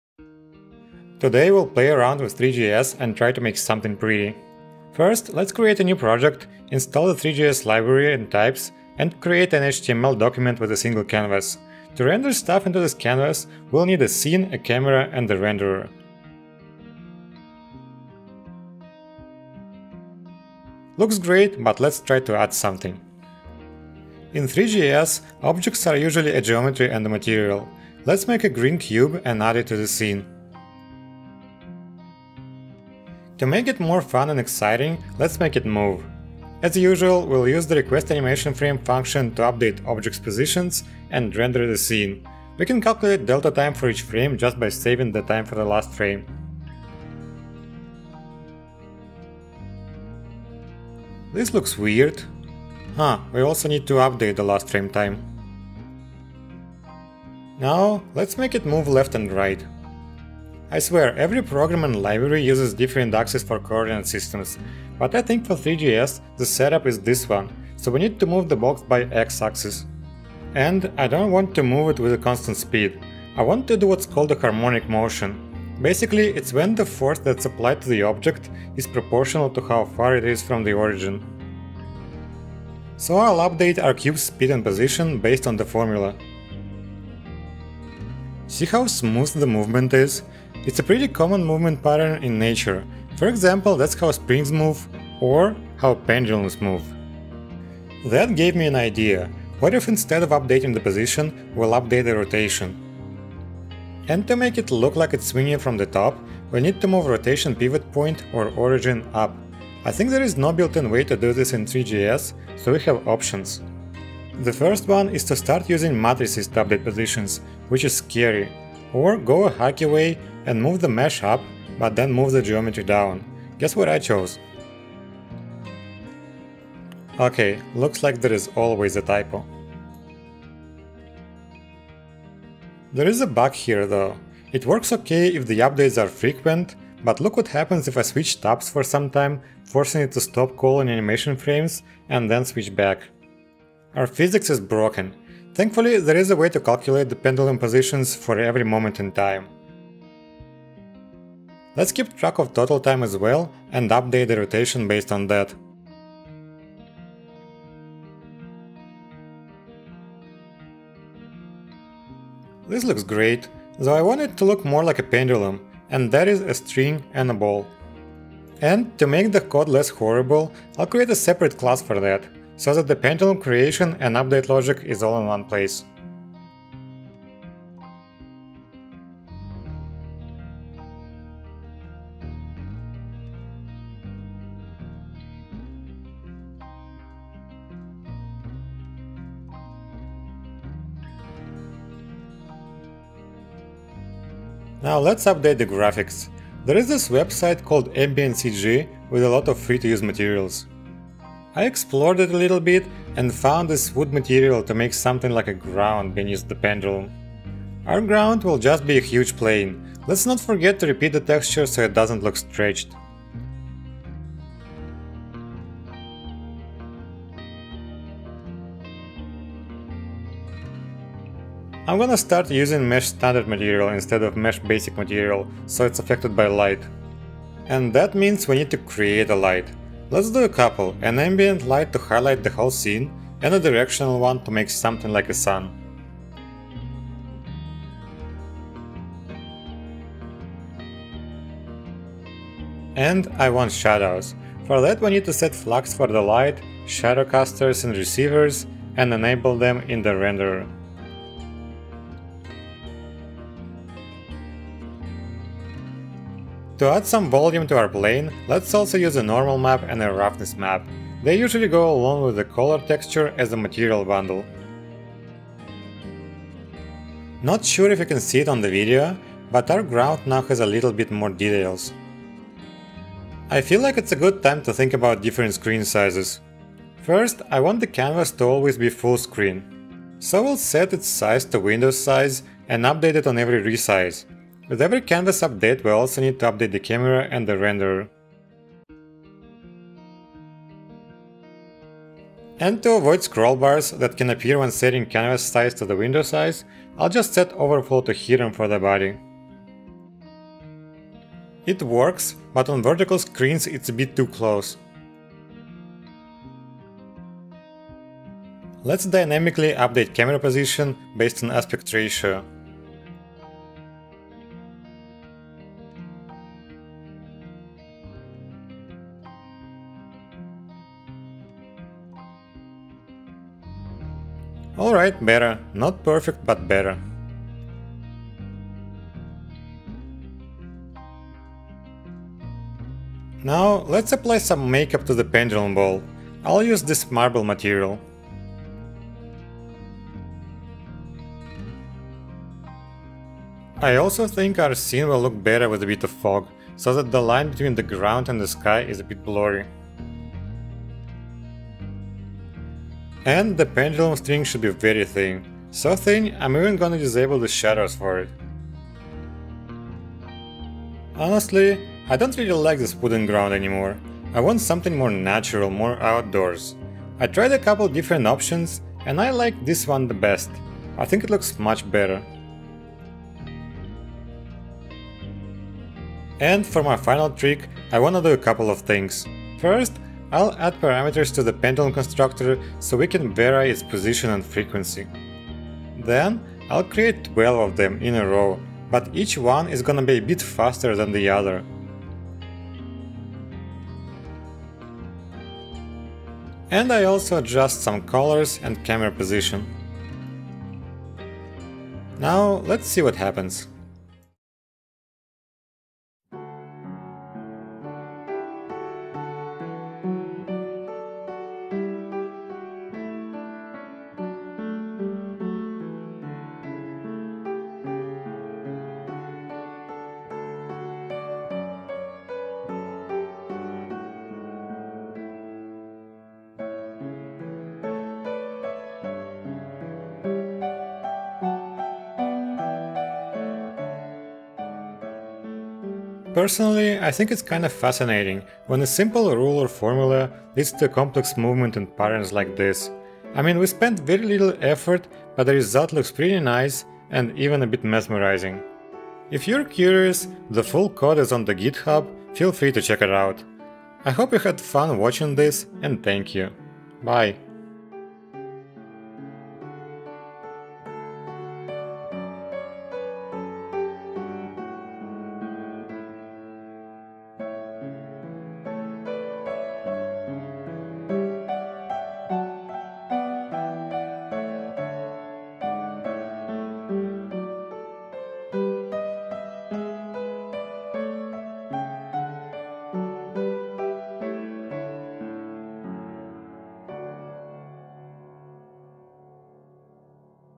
Having fun with pendulums in ThreeJs.
I was messing around with ThreeJs and got an idea of a simple animation.

Tech:
- JavaScript (TypeScript)
- ThreeJs
- Parcel